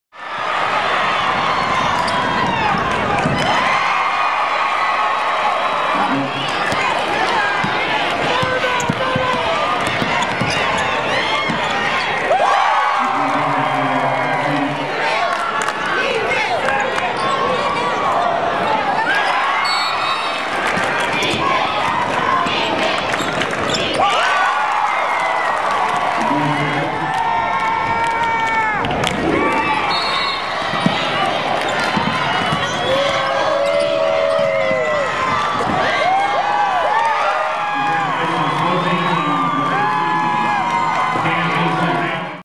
Kansas vs Prague Girls 3-11-11.mp4
Kansas defeated Prague 60-52 to advance to state championship game.